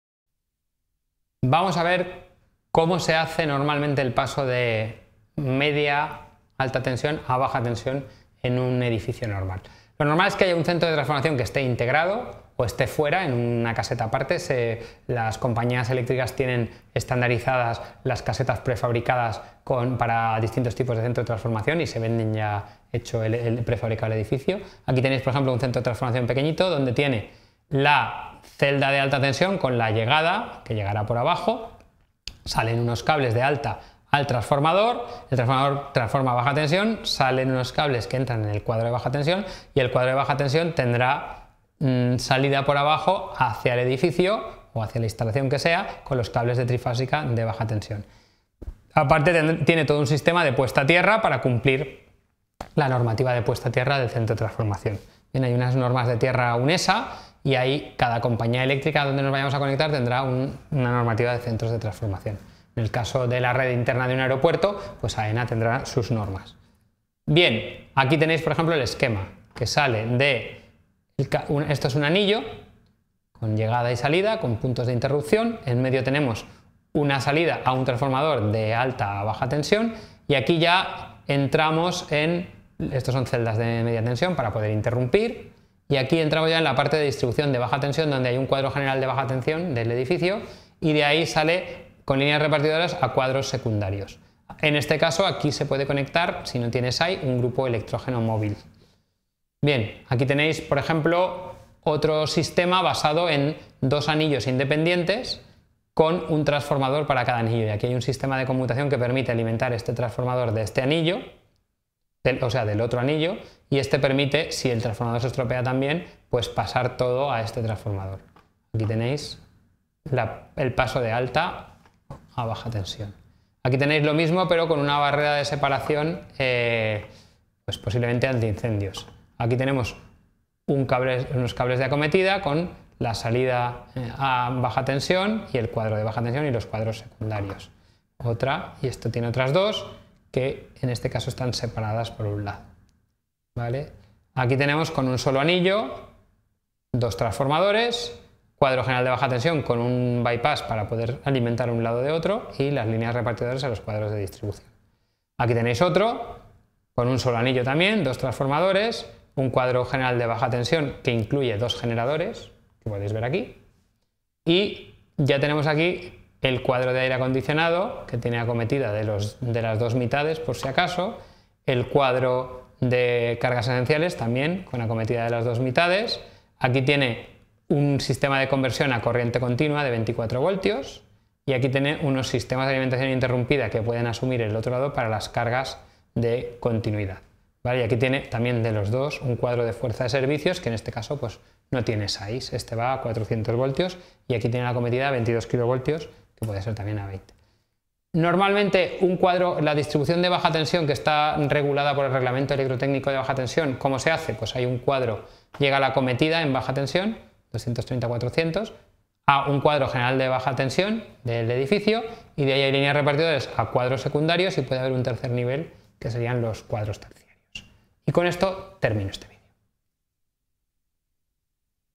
Conversión eléctrica| UPV
Título:  Conversión eléctrica

Descripción automática: En este video se explica el procedimiento estándar para la conversión de energía de media-alta tensión a baja tensión en edificios. Se inicia con la descripción de un centro de transformación, el cual puede ser una estructura integrada o estar ubicada en una caseta separada y prefabricada. Dentro de este centro, se encuentran celdas de alta tensión, transformadores y cuadros de baja tensión que distribuyen la energía al edificio a través de cables trifásicos. Además, se detalla la importancia de un sistema de puesta a tierra que cumpla la normativa vigente.

Se presentan varios esquemas de distribución eléctrica, uno de los cuales incluye un anillo con interrupciones, transformadores alta-baja tensión y la distribución en baja tensión hacia cuadros generales y secundarios. También se mencionan configuraciones con dos anillos independientes, conmutadores para la alimentación entre anillos y sistemas de seguridad como barreras contra incendios.

El video aborda varios tipos de configuraciones con uno o dos anillos, transformadores y generadores, resaltando la posibilidad de conectar un grupo electrógeno en caso de no poseer un sistema de alimentación ininterrumpida (SAI). Se hace mención de sistemas de conversión a corriente continua y la regulación por el Reglamento Electrotécnico de Baja Tensión. Finaliza con el proceso estándar de distribución en baja tensión en edificios, desde un cuadro general hasta cuadros secundarios y terciarios.